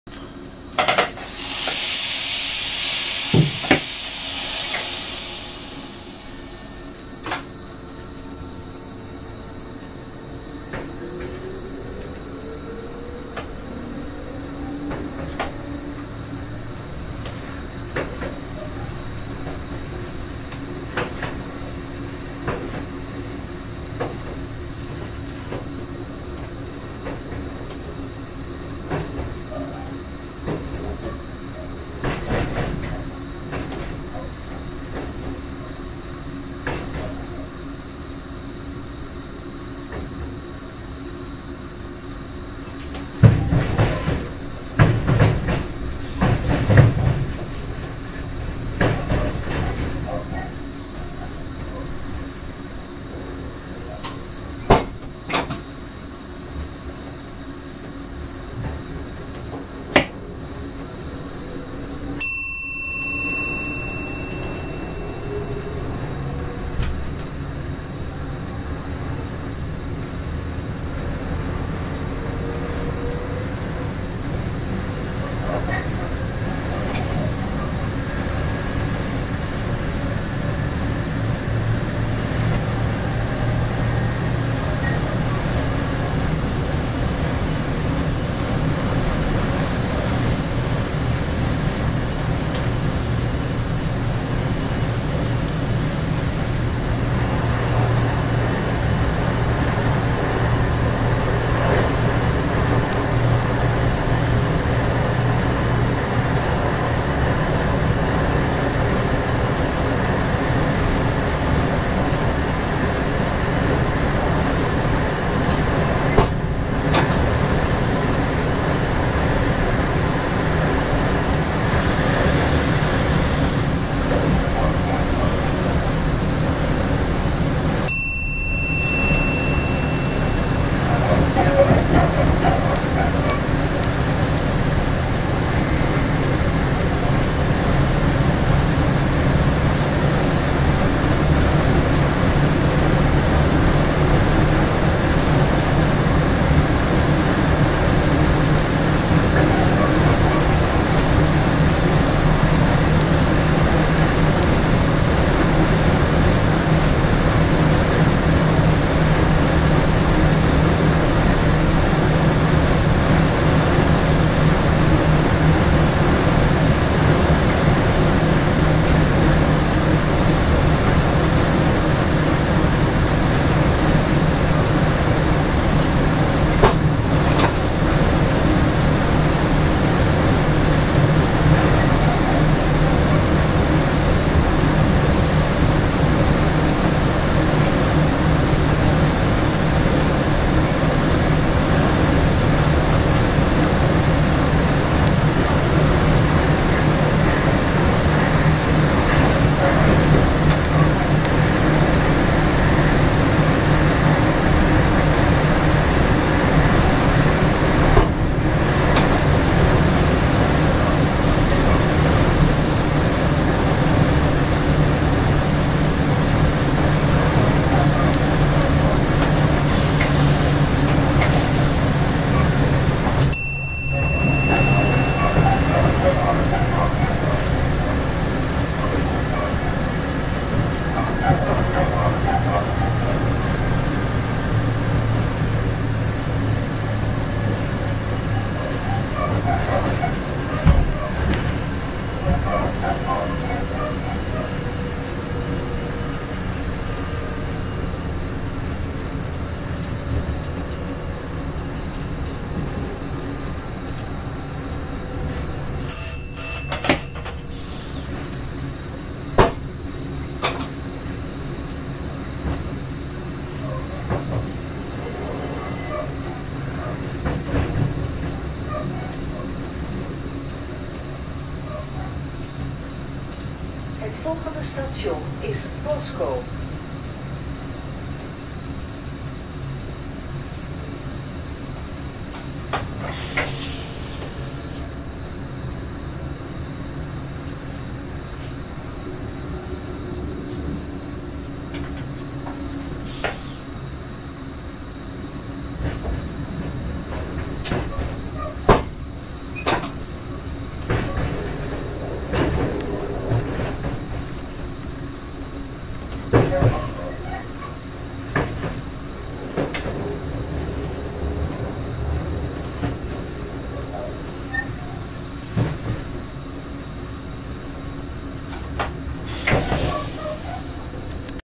03-02-2012 Van Apn naar Bsk het begin van de sneeuwval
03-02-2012 Van Apn naar Bdg het begin van de sneeuwval , nog weinig aan de hand.